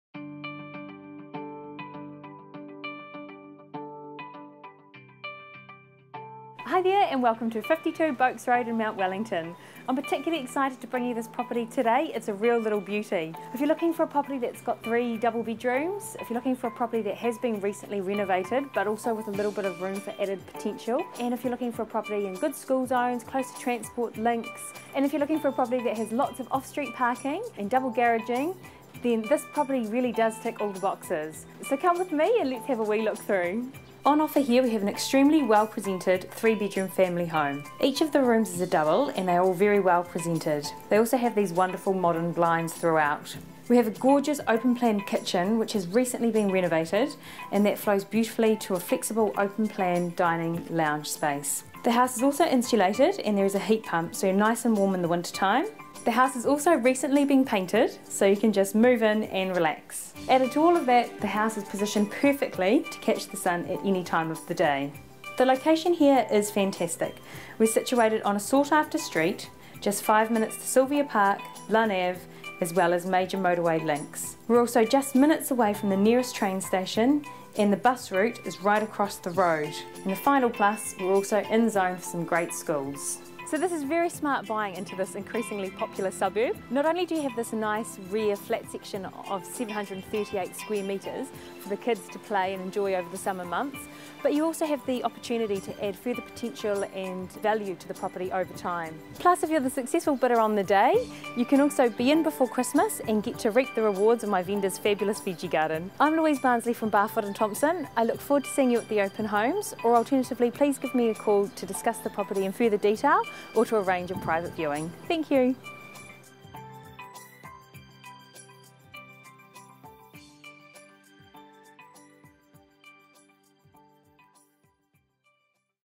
SOLD - 52 Boakes Rd, Mt Wellington - Louise Barnsley
Mt Wellington Beauty

You&#39;ll want to be quick to view this beauty of a property!! Situated in a sought-after street 5 minutes to Sylvia Park, Lunn Ave and motorway access; zoned for good schools; and on a bus ...

Property Marketed by: Louise Barnsley, St Heliers